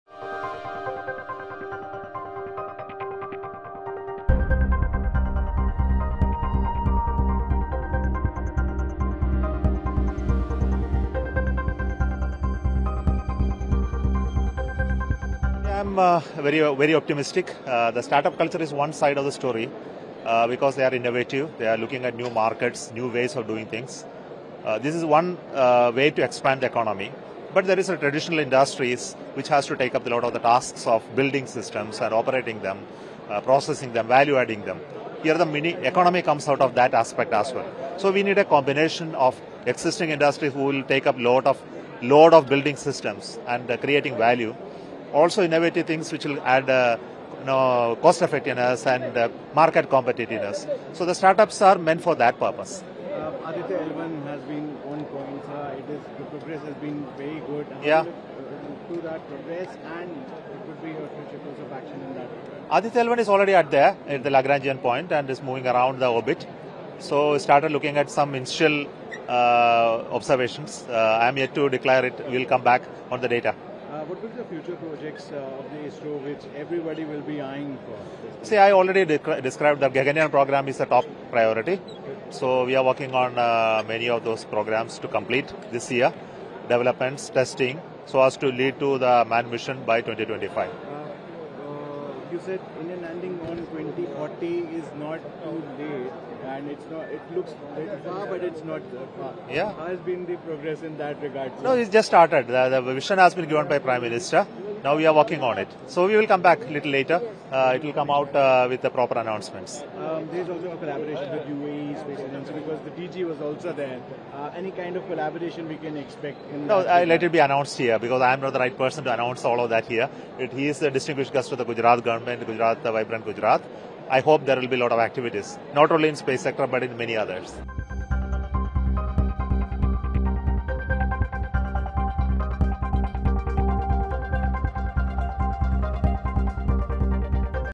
ISRO Chairperson S Somanath makes big announcement on Aditiya L1 mission, Gaganyaan project
During the sidelines of the mega Gujarat Vibrant Global Summit 2024, the Indian Space Research Organisation Chief S Somanath on January 11 made some announcements regarding their future plans that include Gaganyaan mission, India’s moon mission 2040 and AditiyaL1.